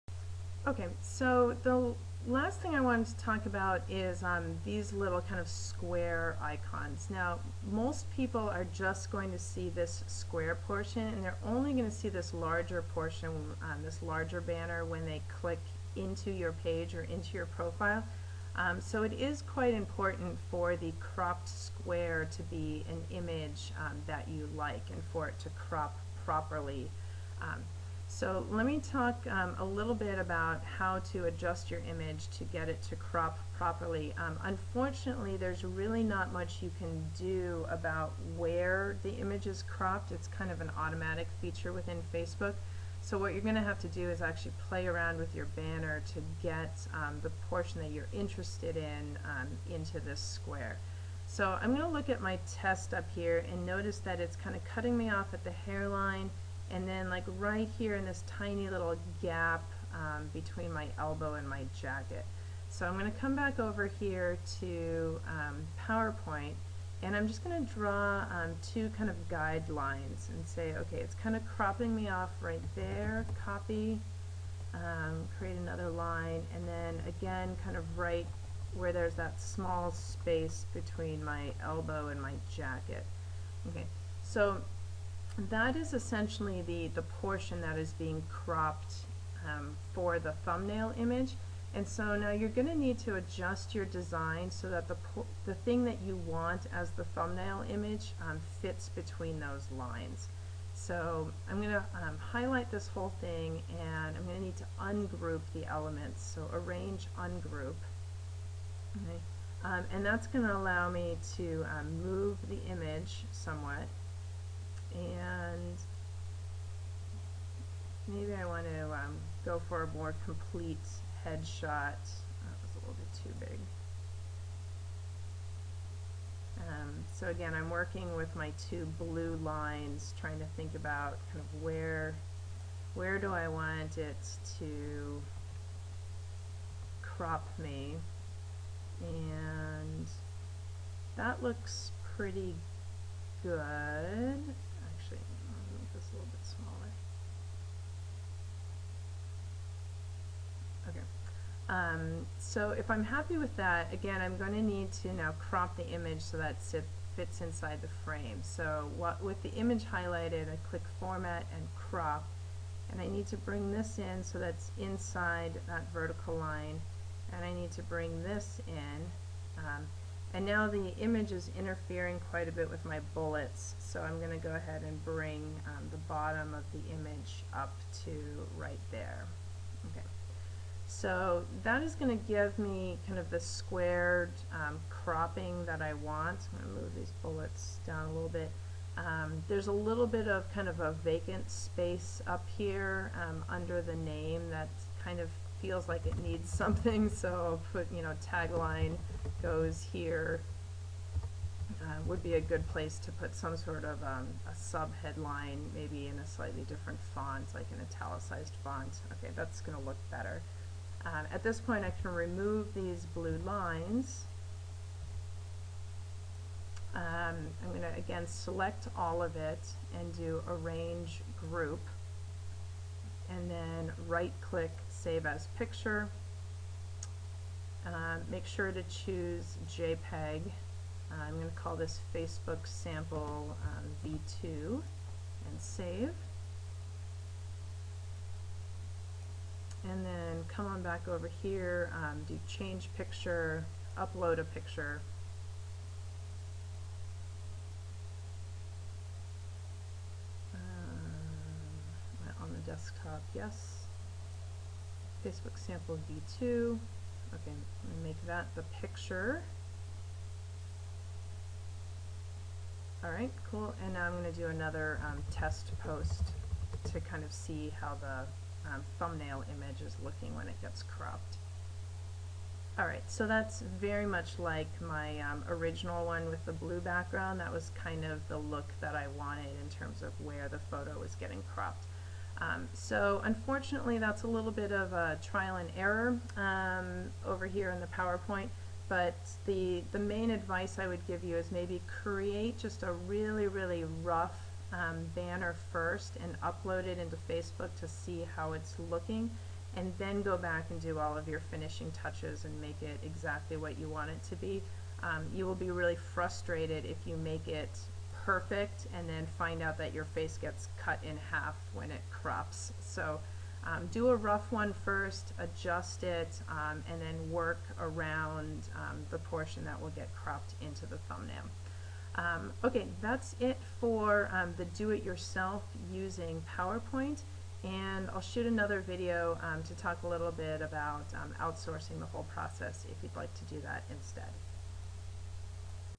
How to Create a Large Facebook Profile Picture (Part 2 of 3)
This video shows you how to create your own good looking large facebook profile picture (200x600) pixels using only Microsoft powerpoint. You can use the profile picture on your personal pages, fan page or both.

Read the blog post and watch the other videos in the series